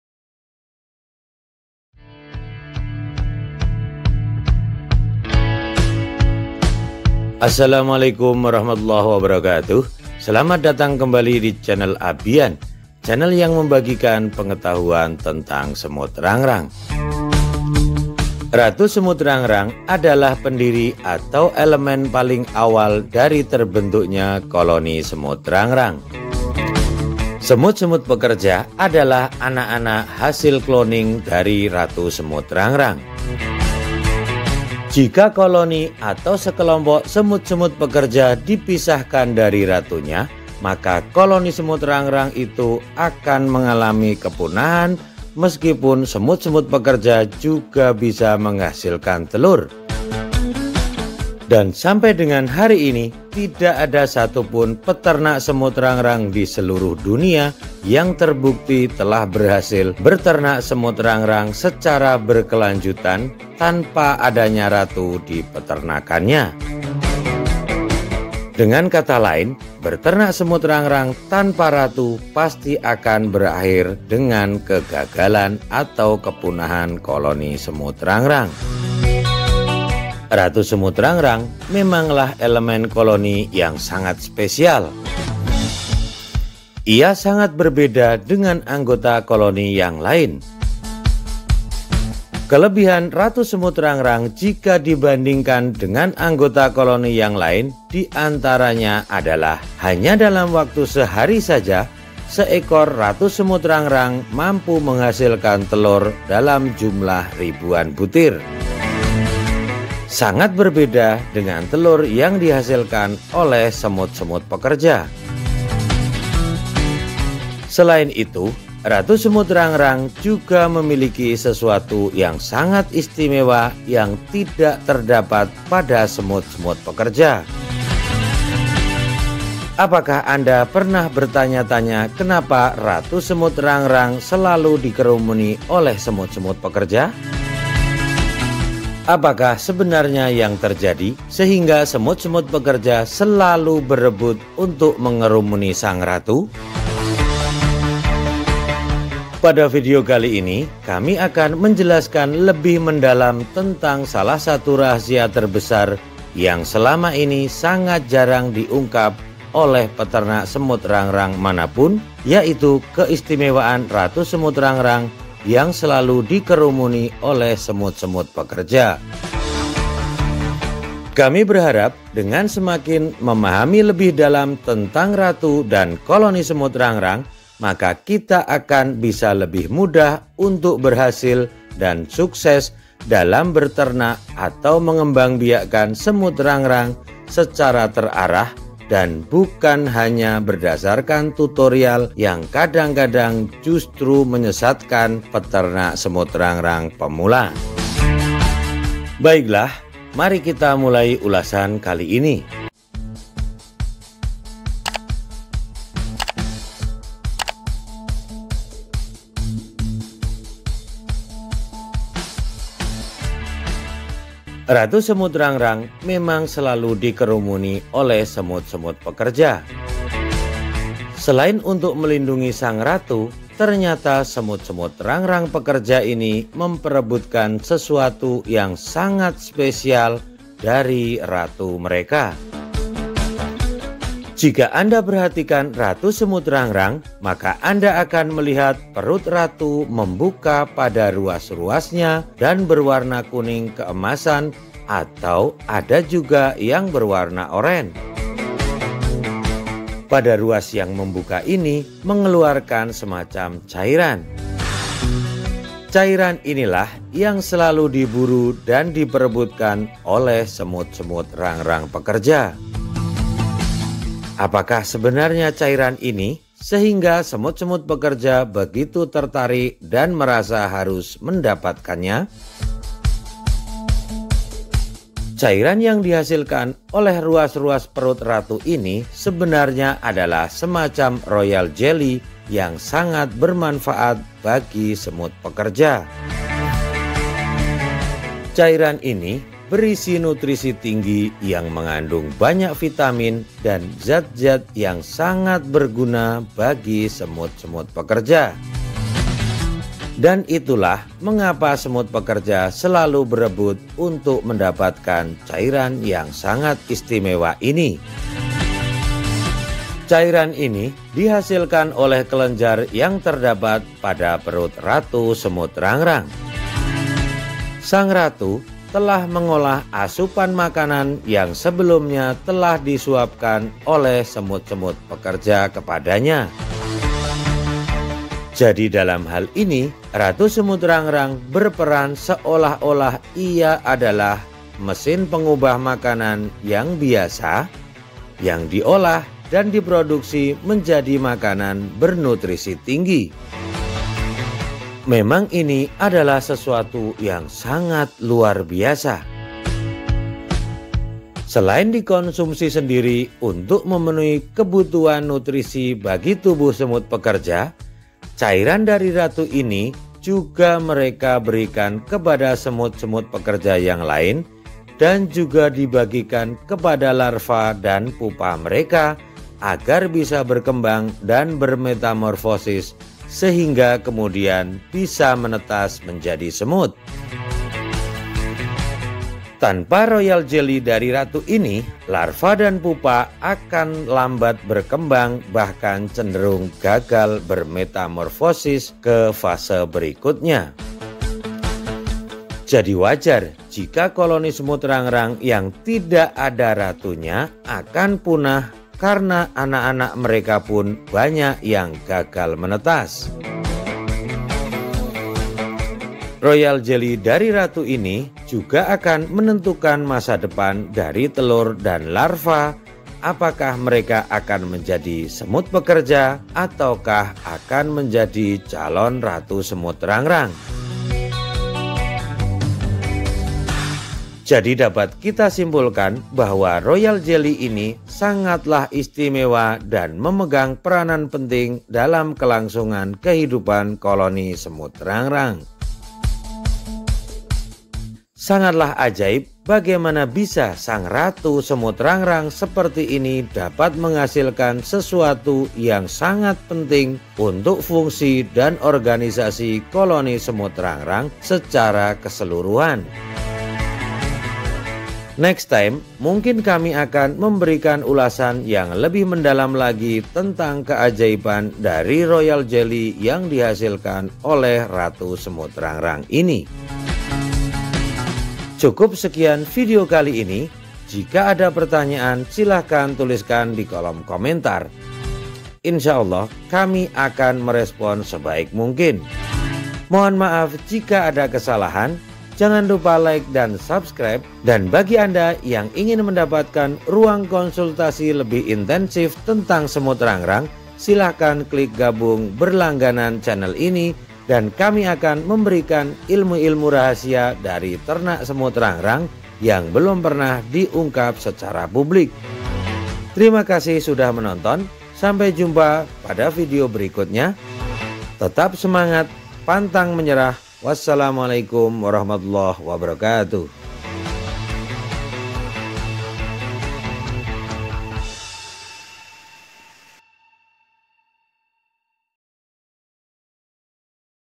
NUTRISI SUPERR DARI RATU SEMUT RANGRANG 🔥🔥🔥
Ratu semut rangrang selalu di kerumuni oleh para semut-semut pekerja.
Apakah sebenarnya yang dilakukan semut pekerja itu ?
Dan apa yang menarik dari ratu semut rangrang sehingga para semut rangrang tidak pernah membiarkan ratu semut sendirian?
Vidio ini akan memberikan ulasan tentang mengapa ratu semut rangrang selalu dikerumuni semut pekerja.
semoga bermanfaat

This video explains why the weaver ant queen is always surrounded by worker ants

There are many language subtitles available in this video please contribute free to translate this video into other languages thanks